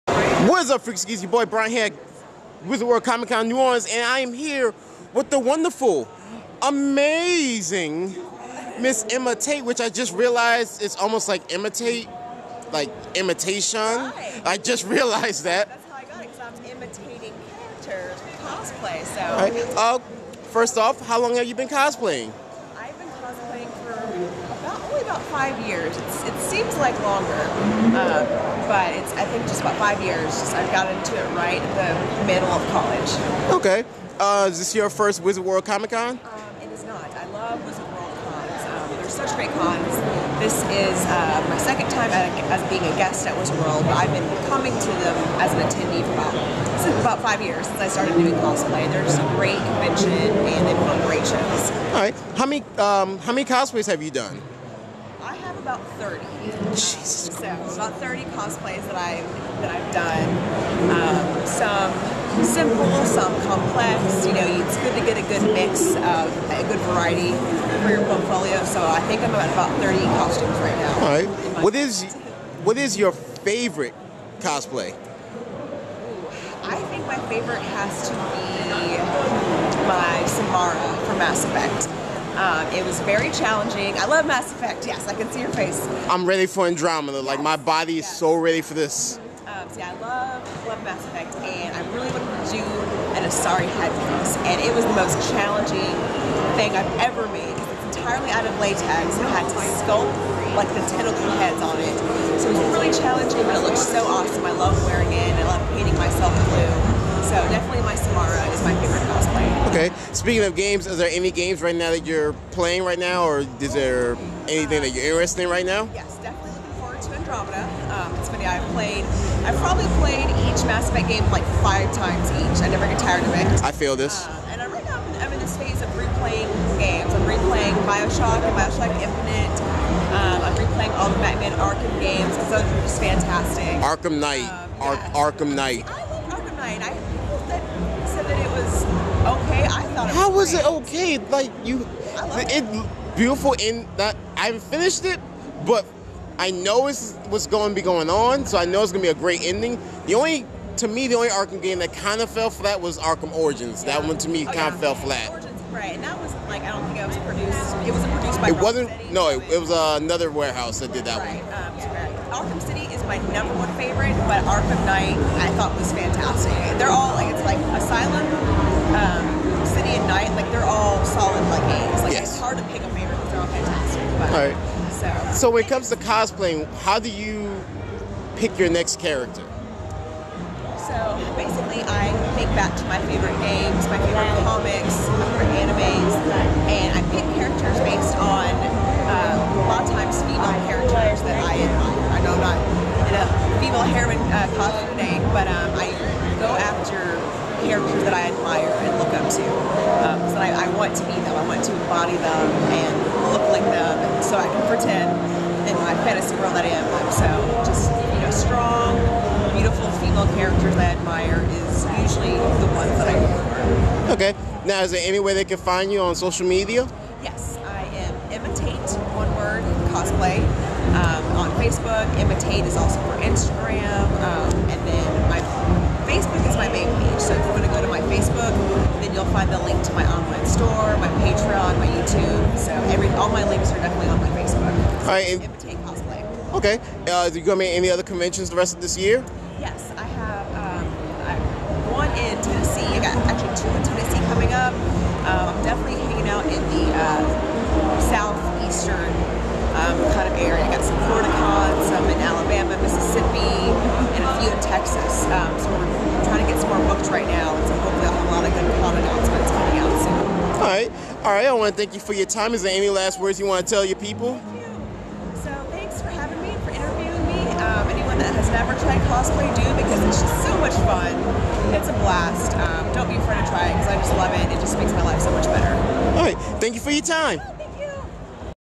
Interview With Cosplayer Emma Tate
Hey Freaks and Geeks while at Wizard World Comic Con New Orleans we had the time to stop and chat with cosplayer Emma Tate we talked a bit about her cosplays and what she likes to play (we're both ready for mass effect to come out by the way) and her favorite cosplays go by her facebook and check here out.